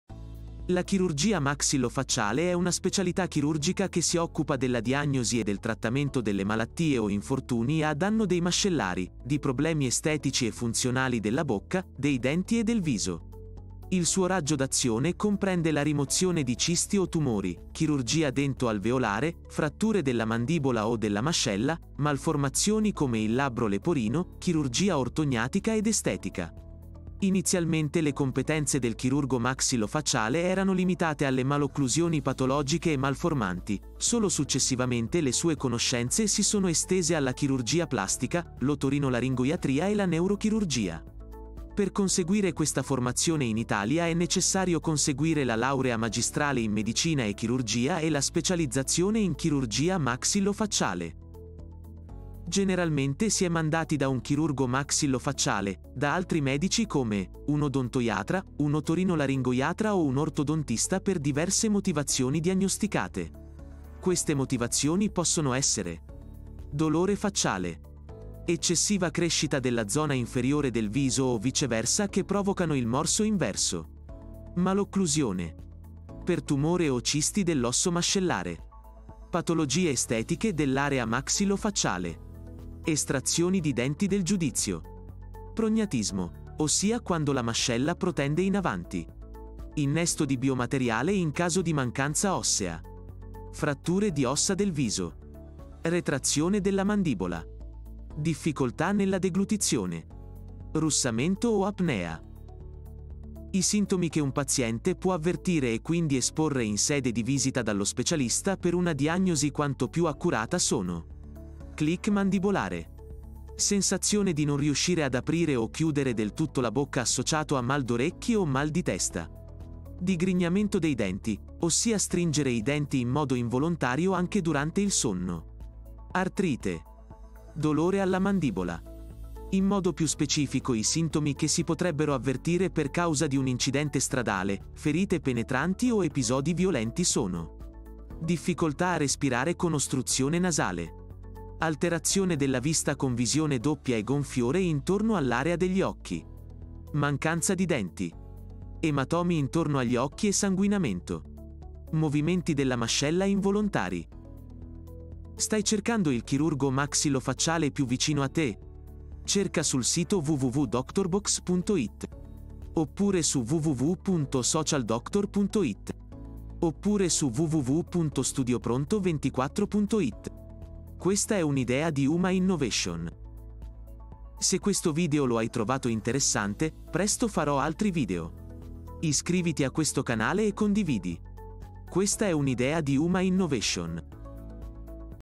Chirurgo maxillo facciale 👩‍⚕️, quando ne hai bisogno? ⚕️ Spiegato velocemente.
COS'È LA CHIRURGIA MAXILLO-FACCIALE?
La Chirurgia maxillo-facciale è una specialità chirurgica che si occupa della diagnosi e del trattamento delle malattie o infortuni a danno dei mascellari, di problemi estetici e funzionali della bocca, dei denti e del viso.

Il suo raggio d'azione comprende la rimozione di cisti o tumori, chirurgia dento-alveolare, fratture della mandibola o della mascella, malformazioni come il labbro leporino, chirurgia ortognatica ed estetica.

Inizialmente le competenze del chirurgo maxillo-facciale erano limitate alle malocclusioni patologiche e malformanti, solo successivamente le sue conoscenze si sono estese alla chirurgia plastica, l'otorinolaringoiatria e la neurochirurgia.

Per conseguire questa formazione in Italia è necessario conseguire la laurea magistrale in Medicina e Chirurgia e la specializzazione in chirurgia maxillo-faccciale.

QUANDO RIVOLGERSI AD UN CHIRURGO MAXILLO-FACCIALE?
Generalmente si è mandati da un Chirurgo maxillo-facciale da altri medici come un Odontoiatra, un Otorinolaringoiatra o un Ortodontista per diverse motivazioni diagnosticate come:

Dolore facciale
Eccessiva crescita della zona inferiore del viso o viceversa che provocano il morso inverso
Malocclusione
Per tumore o cisti dell'osso mascellare
Patologie estetiche dell'area maxillo-facciale
Estrazioni di denti del giudizio
Prognatismo, ossia quando la mascella protende in avanti
Innesto di biomateriale in caso di mancanza ossea
Fratture di ossa del viso
Retrazione della mandibola
Difficoltà nella deglutizione
Russamento o apnea
chirurgia maxillo facciale

HAI I SEGUENTI SINTOMI?
I sintomi che un paziente può avvertire e quindi esporre in sede di visita dallo specialista per una diagnosi quanto più accurata sono:

Click mandibolare
Sensazione di non riuscire ad aprire o chiudere del tutto la bocca associato a mal d'orecchi o mal di testa
Digrignamento dei denti, ossia stringere i denti in modo involontario anche durante il sonno
Artrite
Dolore alla mandibola
In modo più specifico i sintomi che si potrebbero avvertire per causa di un incidente stradale, ferite penetranti o episodi violenti sono:

Difficoltà a respirare con ostruzione nasale
Alterazione della vista con visione doppia e gonfiore intorno all'area degli occhi
Mancanza di denti
Ematomi intorno agli occhi e sanguinamento
Movimenti della mascella involontari